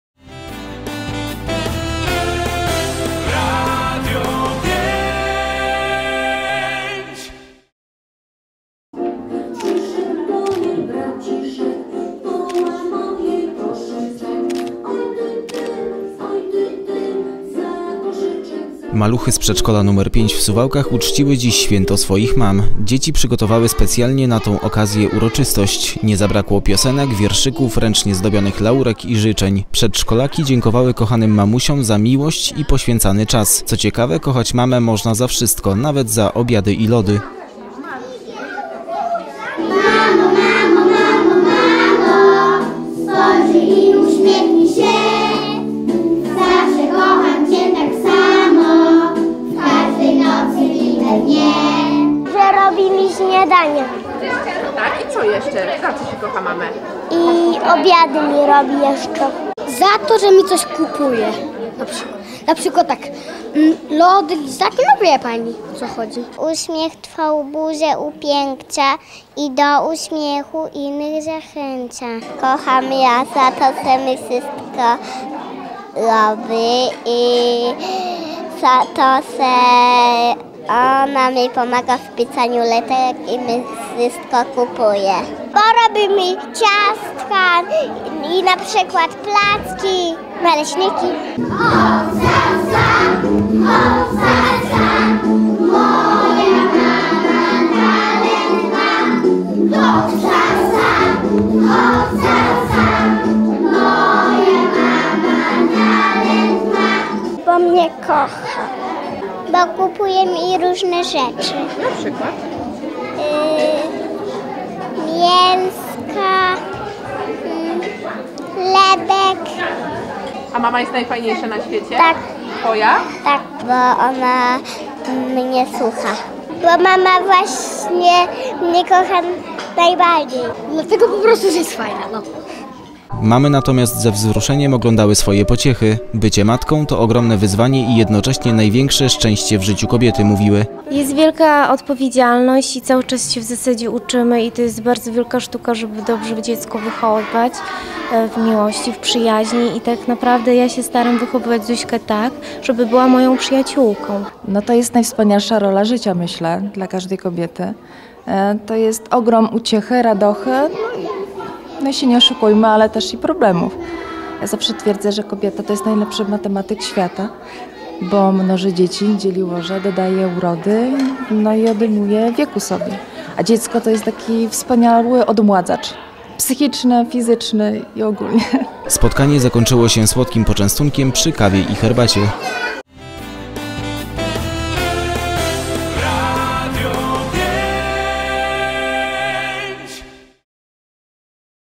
Maluchy uczciły święto mam
Maluchy z Przedszkola nr 5 w Suwałkach uczciły dziś święto swoich mam. Dzieci przygotowały specjalnie na tą okazję uroczystość. Nie zabrakło piosenek, wierszyków, ręcznie zrobionych laurek i życzeń. Przedszkolaki dziękowały kochanym mamusiom za miłość i poświęcany czas. Co ciekawe kochać mamę można za wszytko, nawet za obiad i lody. Mamy natomiast ze wzruszeniem oglądały swoje pociechy. - Bycie matką to ogromne wyzwanie i jednocześnie największe szczęście w życiu kobiety - mówiły. Spotkanie zakończyło się słodkim poczęstunkiem przy kawie i herbacie.